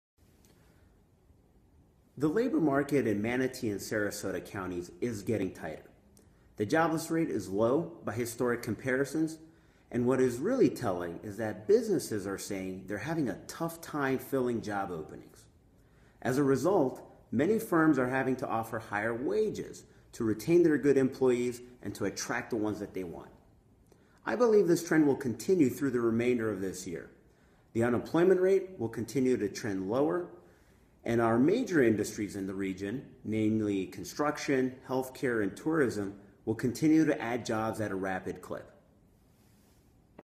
Michael Teshone of The PNC Financial Group speaks about Florida's employment outlook
By: brianries2
Published on: June 9, 2017